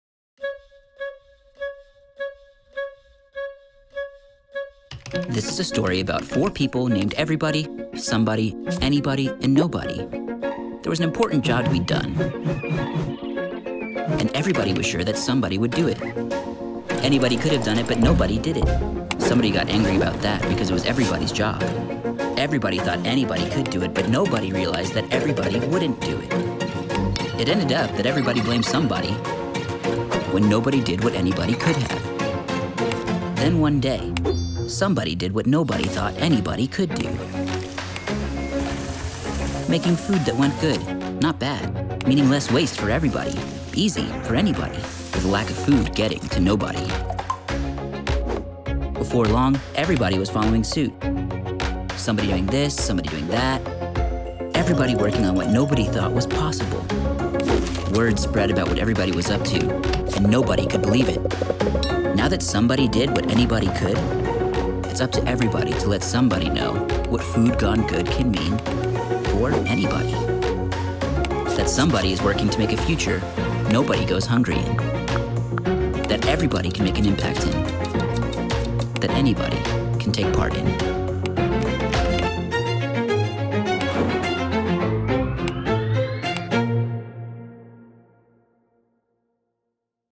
Food Gone Good
What if instead of going bad... food went good?

This is the question that started our story, inspiring us to reduce waste by extending the life of produce for twice as long. So that our food could actually save resources like water and energy, fight climate change, and start a chain reaction inside the food supply chain.

This is a story that is only just beginning. A story that Anybody can take part in and Everybody can impact.

This is the story of Food Gone Good.

Currently, around half of all food grown ends up in a landfill. And we will soon reach a critical point in our ability to feed the world. Apeel looked to nature and found a solution: longer-lasting produce. From fresh avocados to limes, Apeel adds a “little extra peel” that helps to extend the shelf life of fresh produce and reduce the amount of food gone to waste.

Made from plants, Apeel works with nature to improve access to fresh produce, reduce waste, and build a sustainable, abundant future for all.